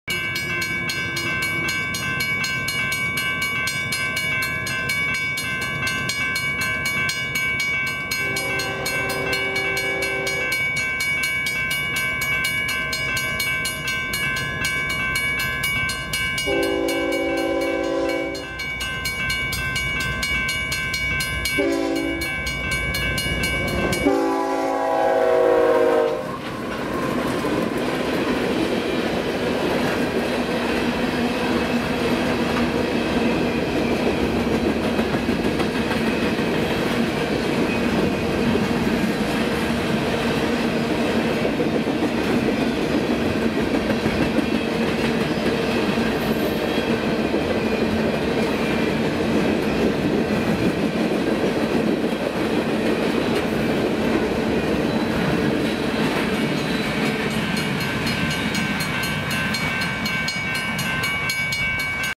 UP Golden State Route: returning Herzog work train 07/13/2018
EB returning Herzog work train at sunrise

UP GEVO # 8027, UP C44AC # 7102 repaint

MP 299.6/UP Pratt Sub.  Pratt, Kansas